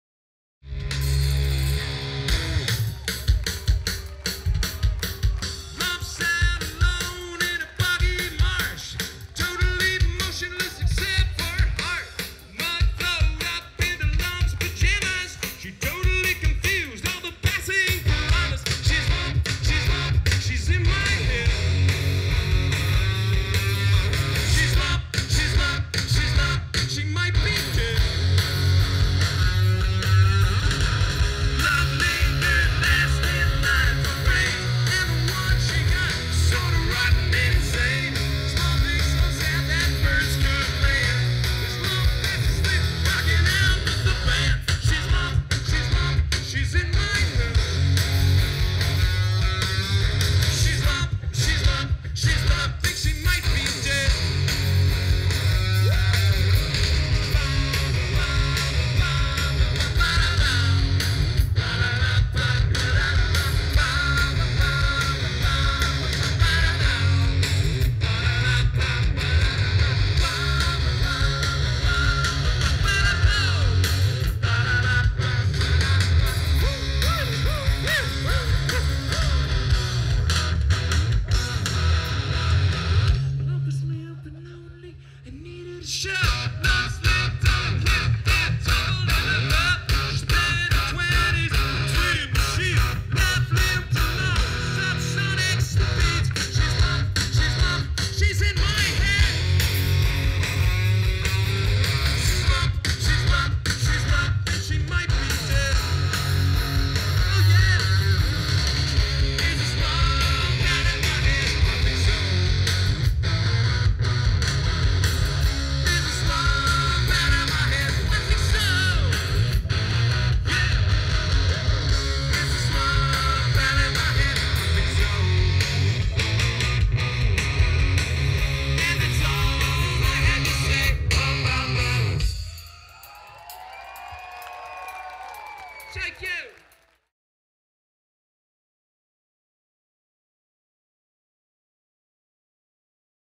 2009 North Park Music Thing - Presidents of the United States of America
The North Park Music Thing - San Diego Music & Media Conference and Festival is a three day music event, which will take place August 7-9, 2009 at the Lafayette Hotel & Suites, 2223 El Cajon Blvd. · North Park • San Diego, CA.